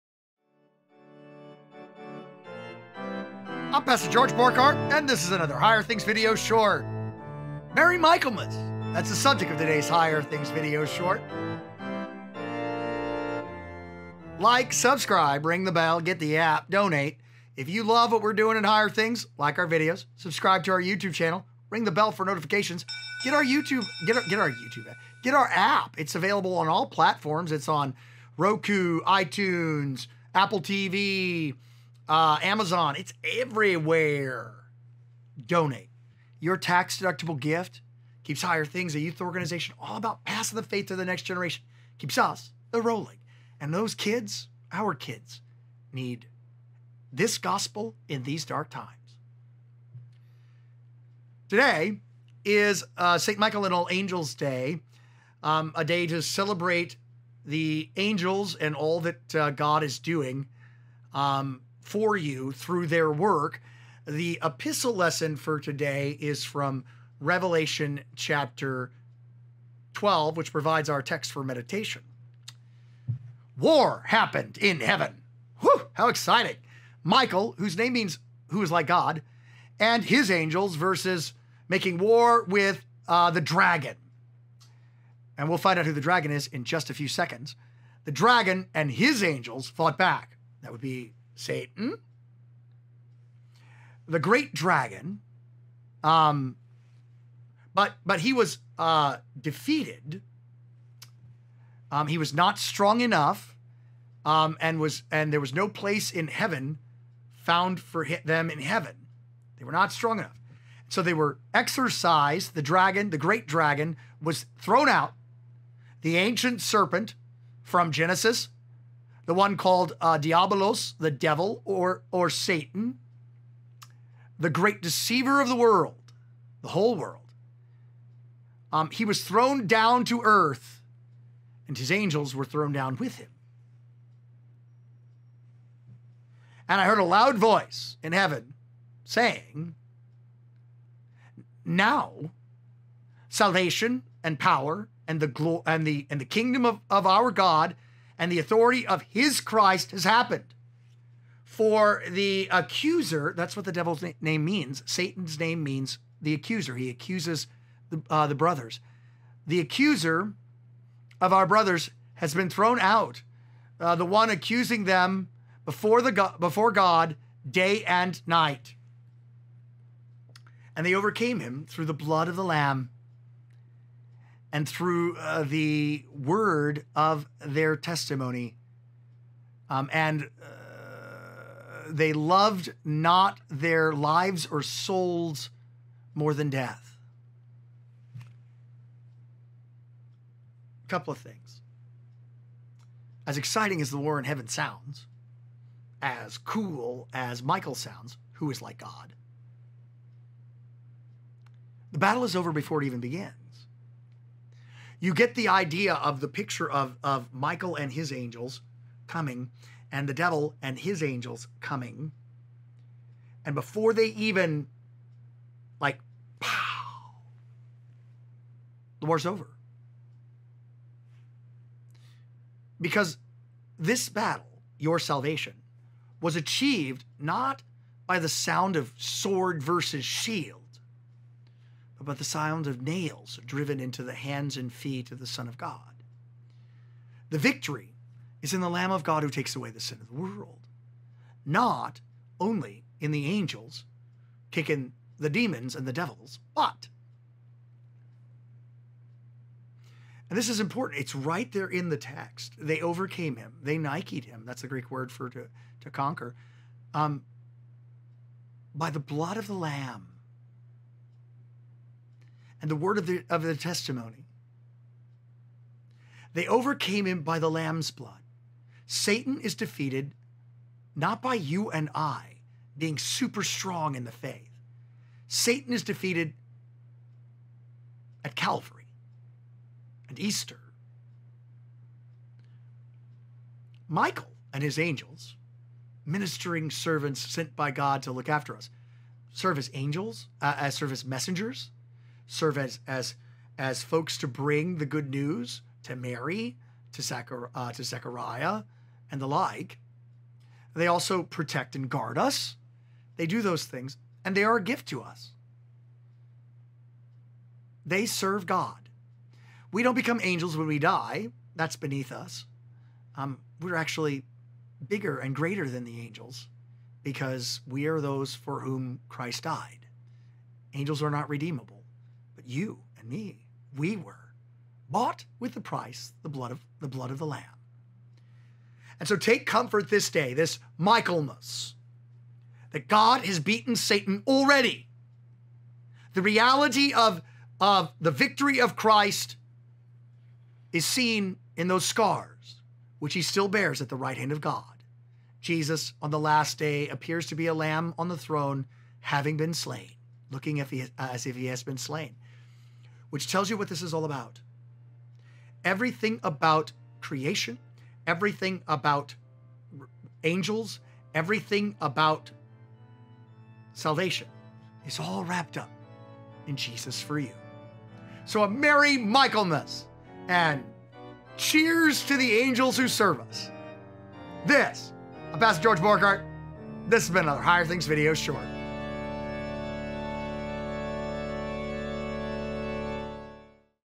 0929 Michaelmas and The War In Heaven - A Higher Things® Video Short
Merry Michaelmas! As exciting as the war in heaven is and sounds, the battle was over before it even began. They conquered through the blood of the Lamb. Satan is defeated at Calvary and Easter.